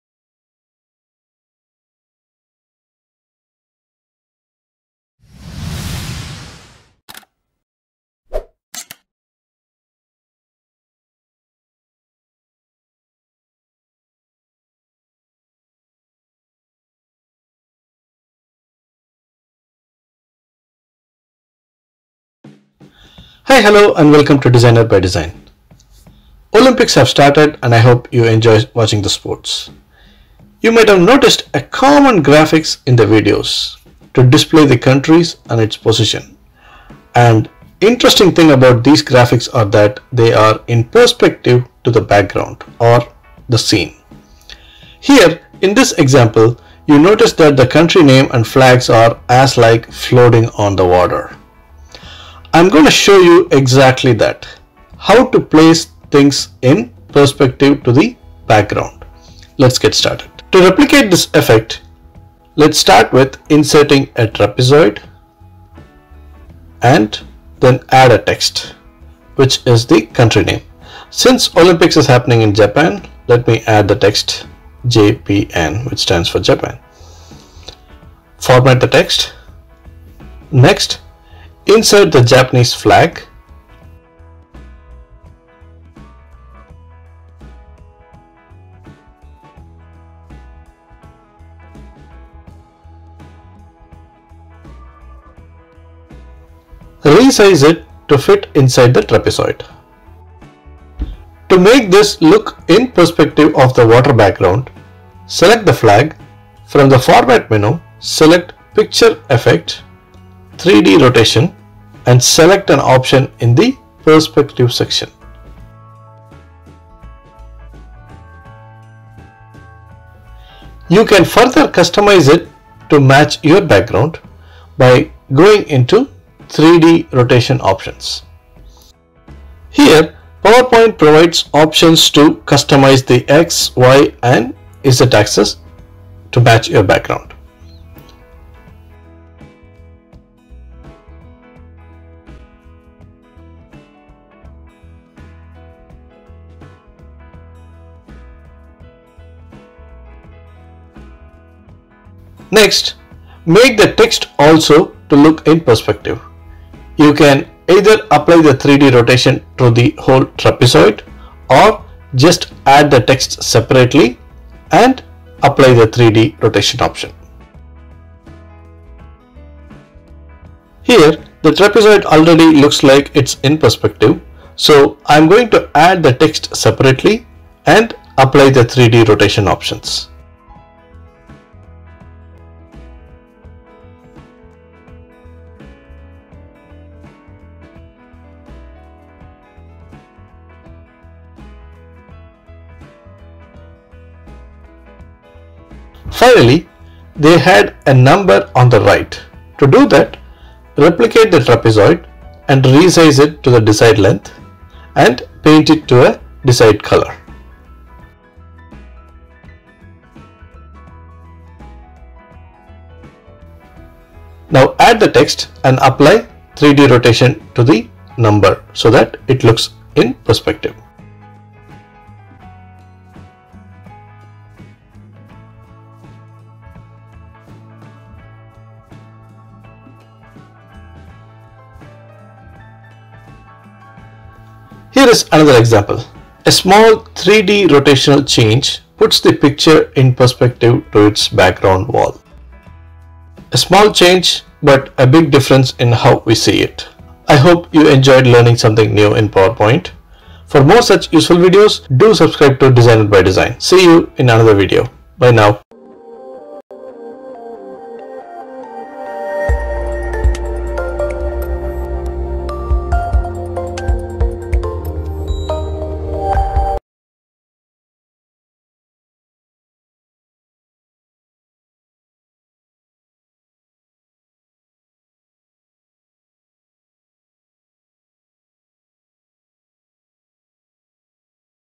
How do they do these Effects. PowerPoint Hacks Simplified.
How do they make this effect? Placing text and images in perspective to the background

00:00 Intro
01:00 Setup Text and Image
01:50 Set Images in perspective to background
02: 50 Set Text in perspective to background
03:55 Examples of setting images on wall in perspective
04:20 Subscribe to Dzyner By Design

Designer By Design Dzyner, Voice, Subtitles by Senthil Kalaimani Simplified PowerPoint Hacks for Design